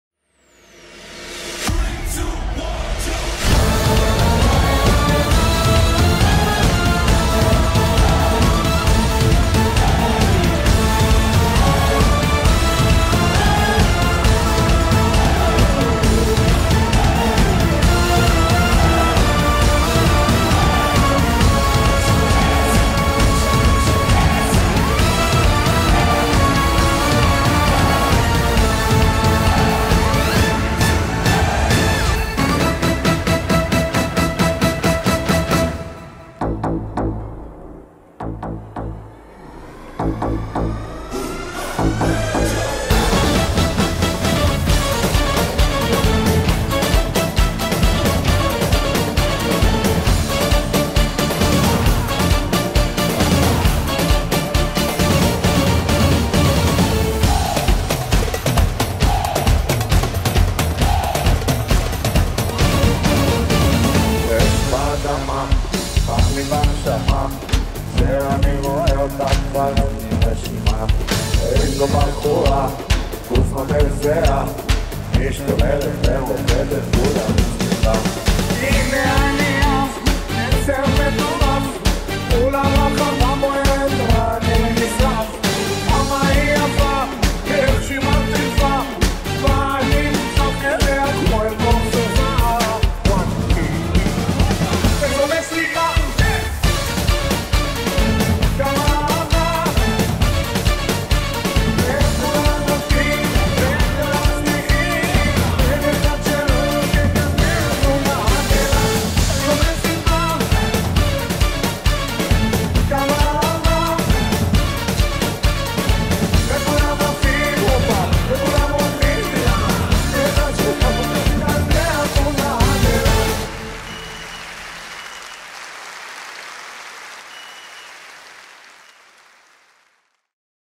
Tararam Group with Omer Adam at Live Park - Rishon Lezion
Artistic Director of Tararam - Doron Raphaeli